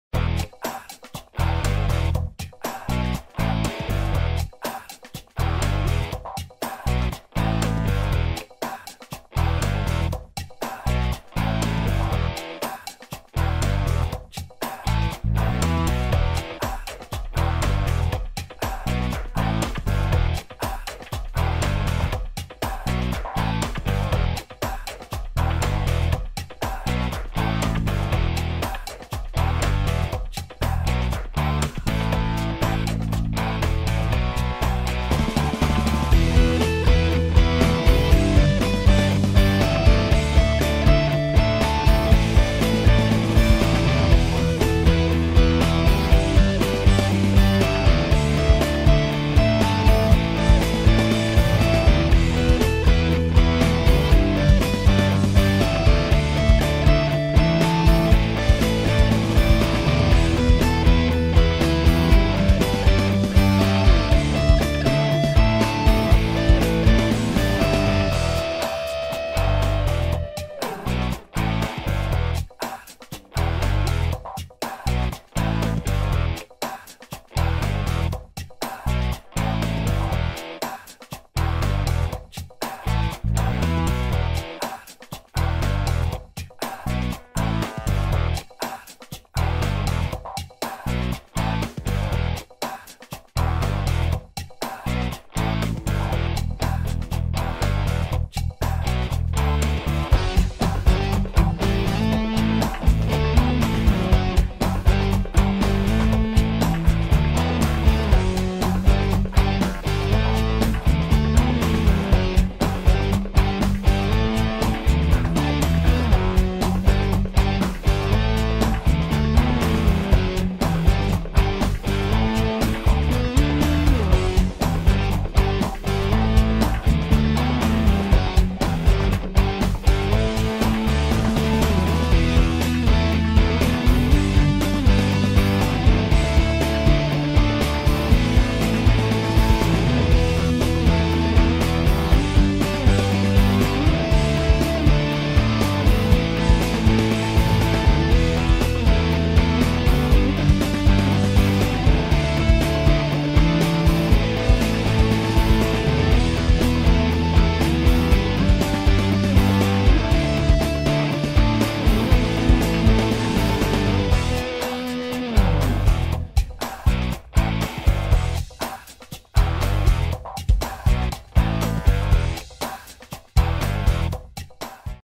Funko Pop Marty and Doc Back to the Future 360 View
Marty in Puffy Vest No.961
Doc 2015 No.960